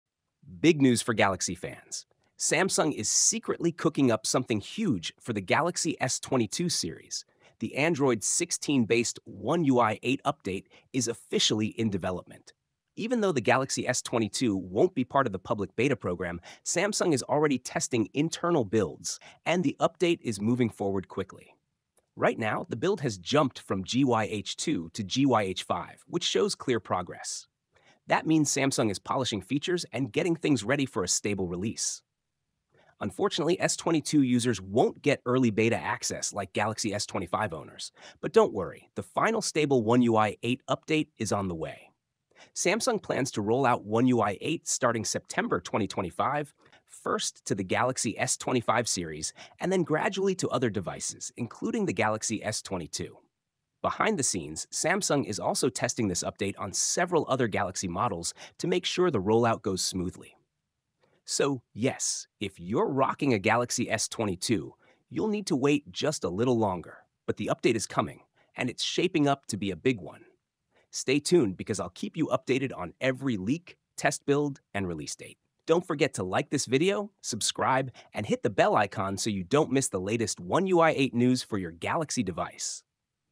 Samsung Galaxy S22 One UI 8 Update - Android 16
Samsung Galaxy S22 One UI 8 Update - Android 16 / Samsung One UI 8 (Android 16) Update for Galaxy S22 Explained

Samsung is officially working on the One UI 8 (Android 16) update for the Galaxy S22 series, and the development is moving forward quickly! The internal test builds for the Galaxy S22 One UI 8 update have already progressed from version GYH2 to GYH5, showing that Samsung is preparing the software for release. While the Galaxy S22 won’t be part of the One UI 8 Beta program, users can expect the stable One UI 8 release in September 2025, starting with the Galaxy S25 series and then rolling out to more devices. This video covers all the latest Galaxy S22 One UI 8 news, release date details, new features, and internal testing leaks. If you’re waiting for Android 16 on the Galaxy S22, Galaxy S22+, or Galaxy S22 Ultra, this is everything you need to know!